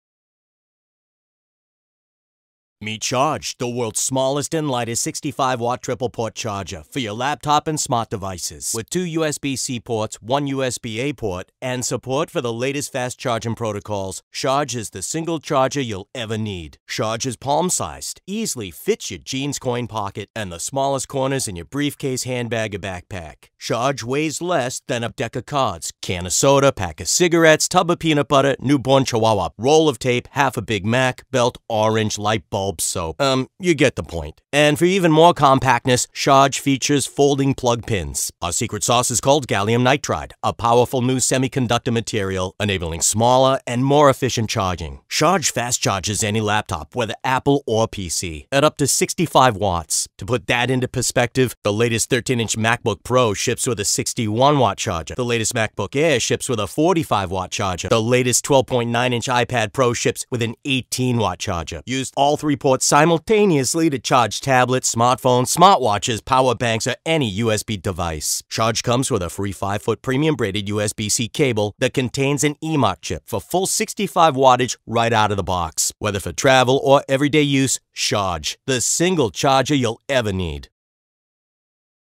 World's Smallest & Lightest Laptop & 3-Port USB Wall Charger
The Single Charger You'll Ever need: With 2 USB-C & 1 USB-A ports, SHARGE juices up your laptop and smart devices simultaneously.